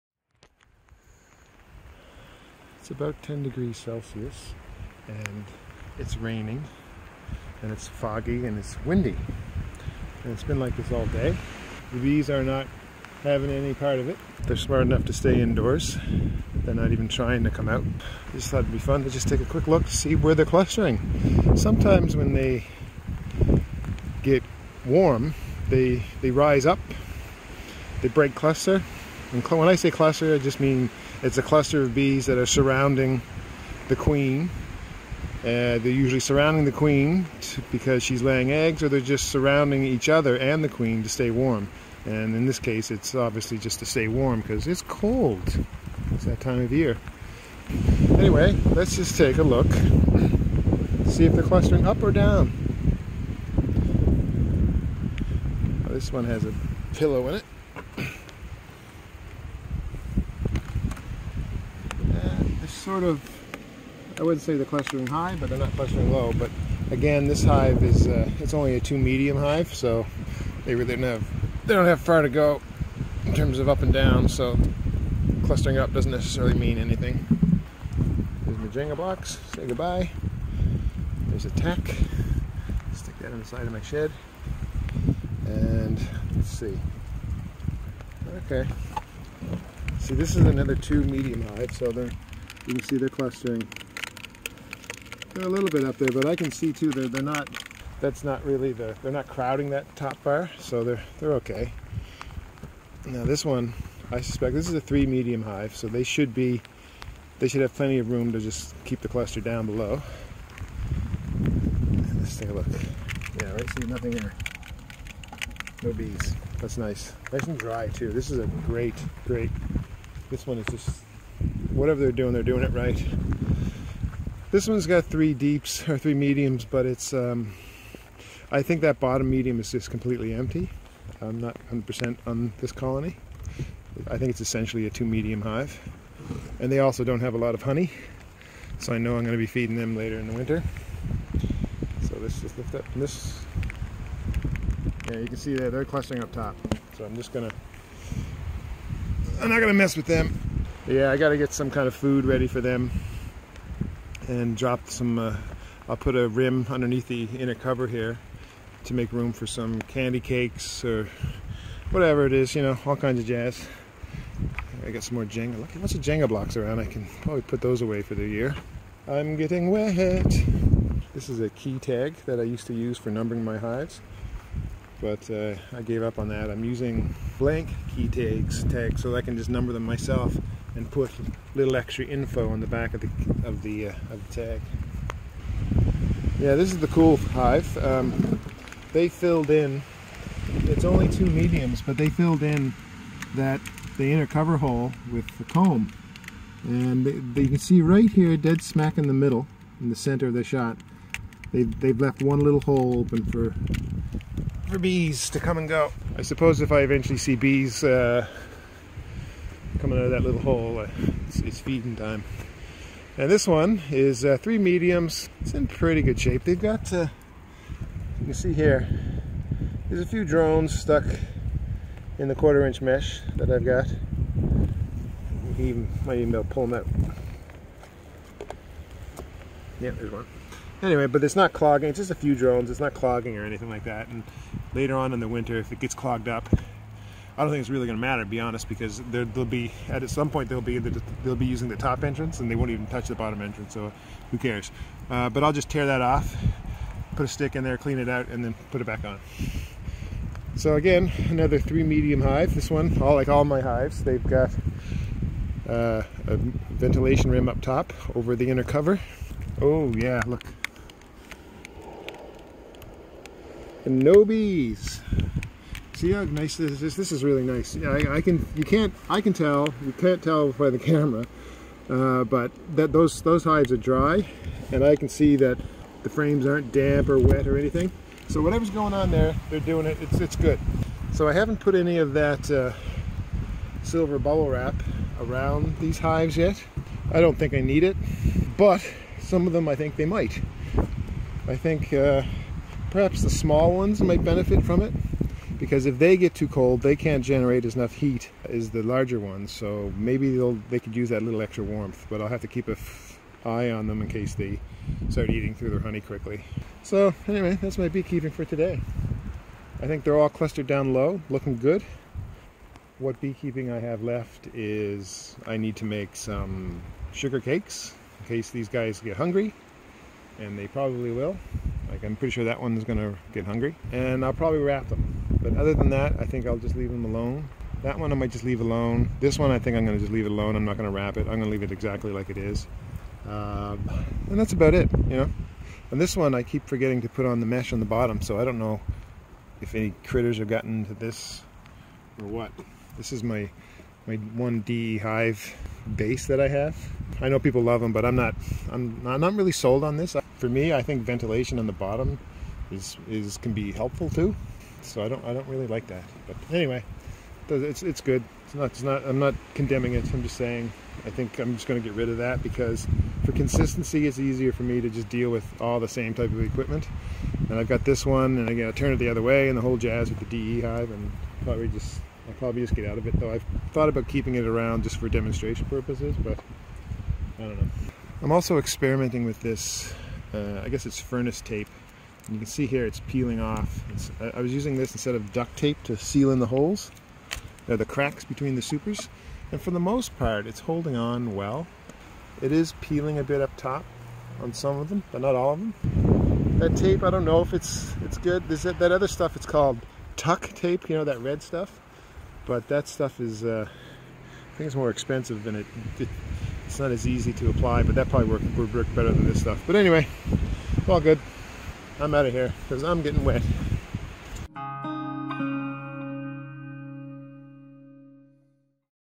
Beekeeping in the Rain (in Newfoundland)
Lifting the hood off a few hives to see how high the bees are clustering in the rain -- in early December in Newfoundland.